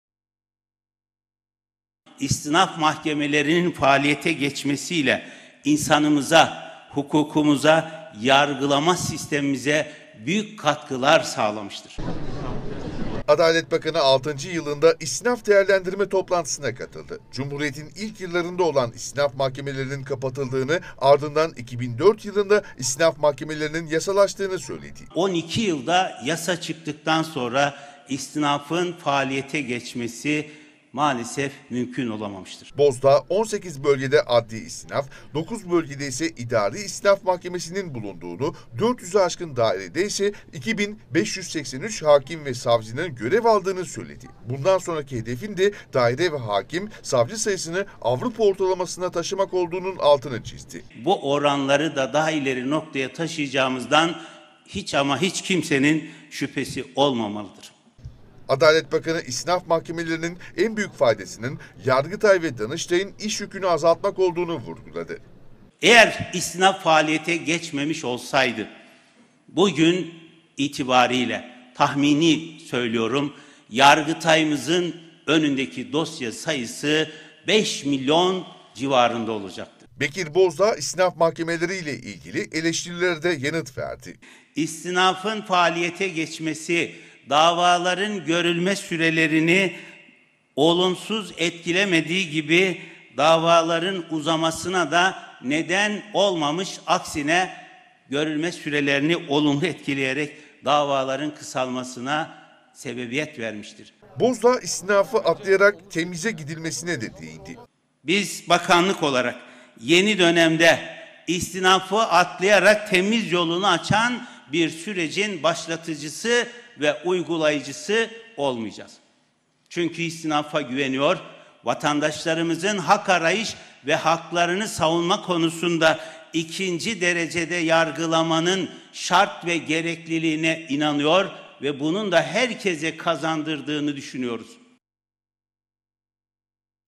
Bozdağ'dan istinaf mahkemeleri açıklaması
Adalet Bakanı Bekir Bozdağ, istinaf mahkemelerinin Danıştay ve Yargıtay’ın iş yükünü azalttığını açıkladı. Bozdağ, “İstinaf faaliyete geçmemiş olsaydı,  Yargıtayımızın önündeki dosya sayısı 5 milyon civarında olacaktı” dedi. Adalet Bakanı İstinaf mahkemelerinin davaların görülme sürelerini uzatmadığını da kaydetti.